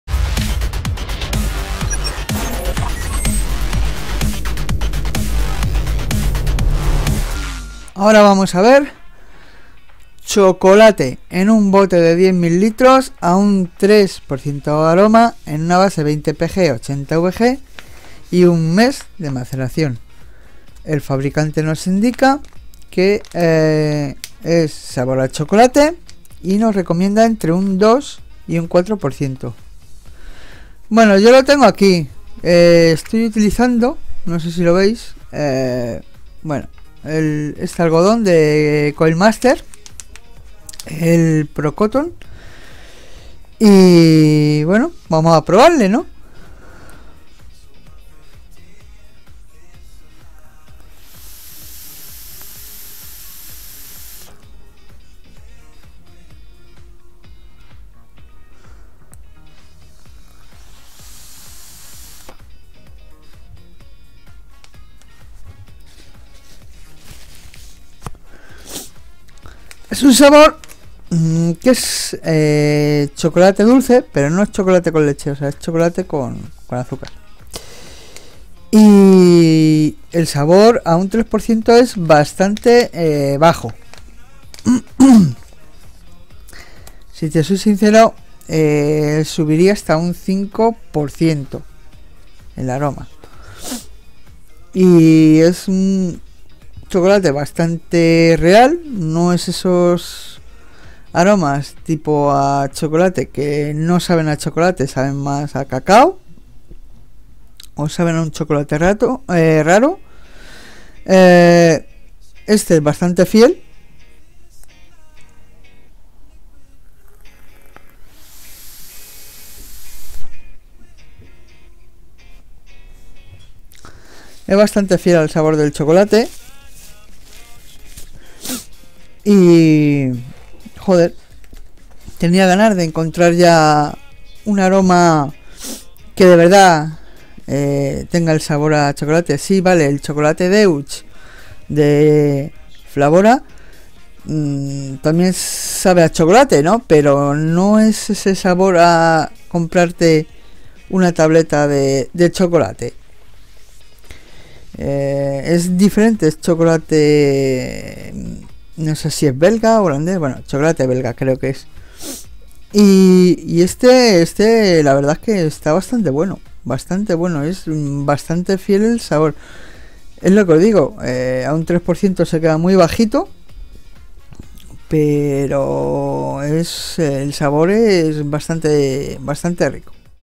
Revisión de aromas Inawera: Chocolate
Hoy revisamos este aroma de chocolate de Inawera, un aroma que tenía ganas de probar ya que me han hablado bien de él.

Este aroma tiene un sabor muy real a chocolate azúcarado, la típica tableta de chocolate que compras en el super. Creo que a un 3% de aroma el sabor no es potente pero si lo subimos a un 5% el sabor mejora mucho.

Aromas que merecen mucho la pena comprar ya que precio calidad al ser ultraconcentrados esta fenomenal y el sabor es excelente.

El fabiricante recomienda:

Tiempo de maceración: 30 días, suponemos que al ser aroma de pastelería ncecesita ese tiempo de maceración ya que el fabricante no dice cuanto tiempo de maceración es necesario para este aroma.

La revisión esta recializada:

Base: 20 PG/80 VG
Porcentaje utilizado: 3 %
Tiempo de maceración: 30 Dias